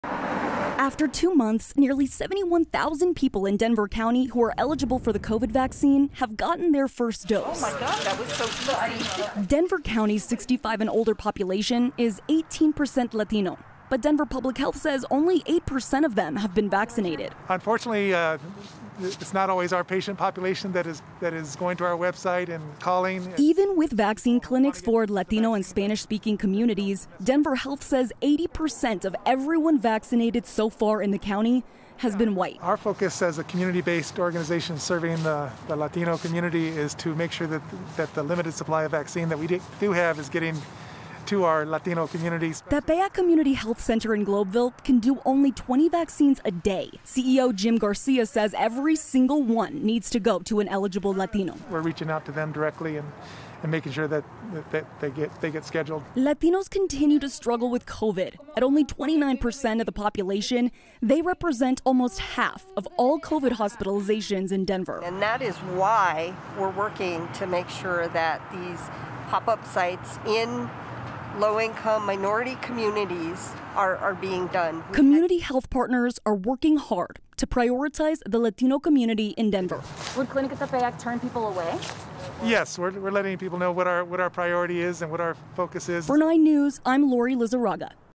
Colorado counties start tracking race and ethnicity to focus on vaccine equity
Denver County's 65 and older population is 18% Latino – but, Denver Public Health said only 8% of them have been vaccinated.

9NEWS (KUSA) is located in Denver, Colorado.